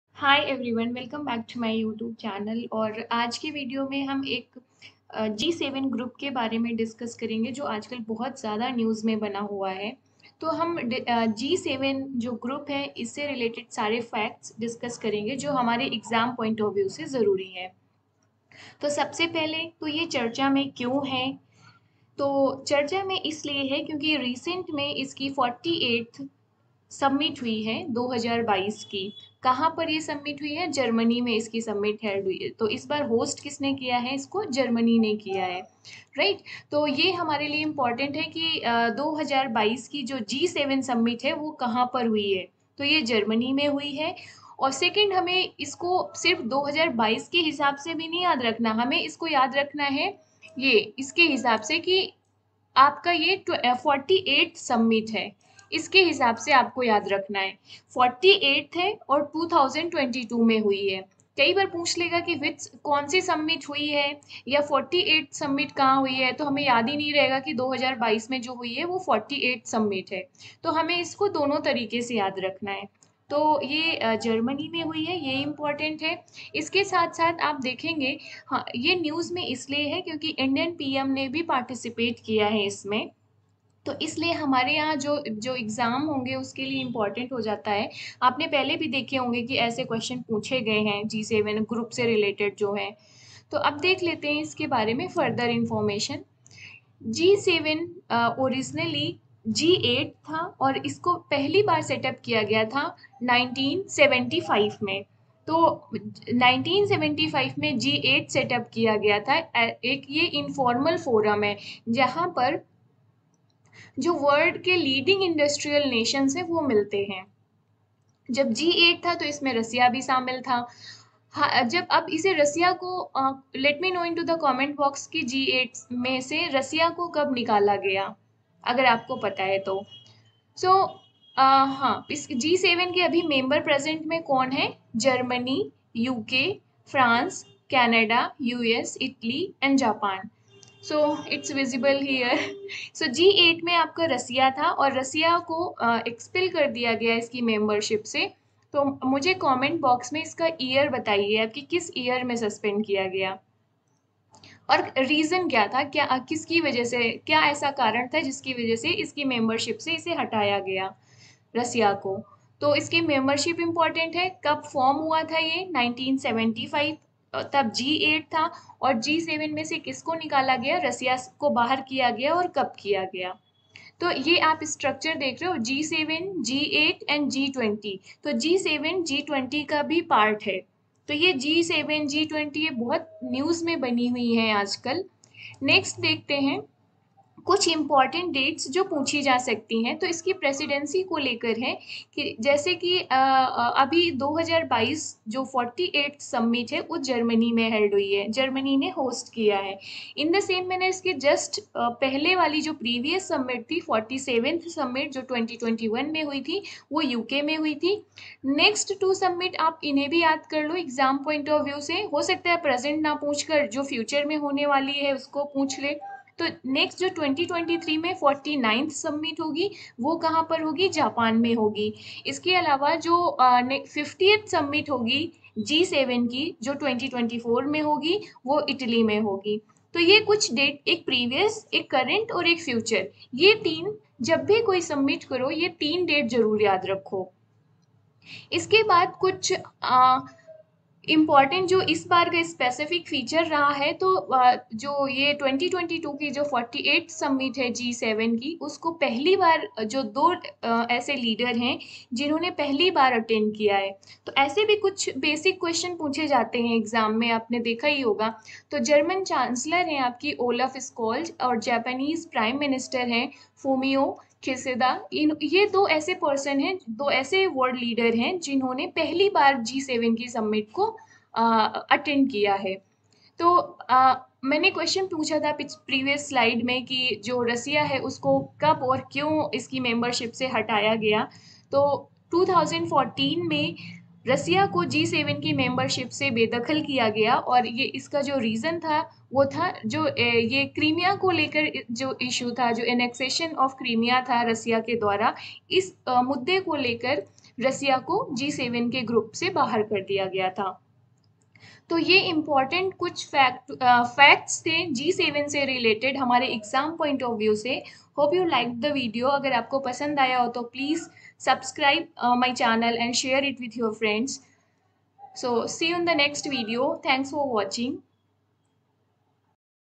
G7 Summit 2022 | Group of 7 Countries | Current Affair Analysis || G7 शिखर सम्मलेन | Key Highlights
In this video we have discussed the G 7 SUMMIT 2022. Current Affair Analysis.

If you like our videos then kindly help us to grow.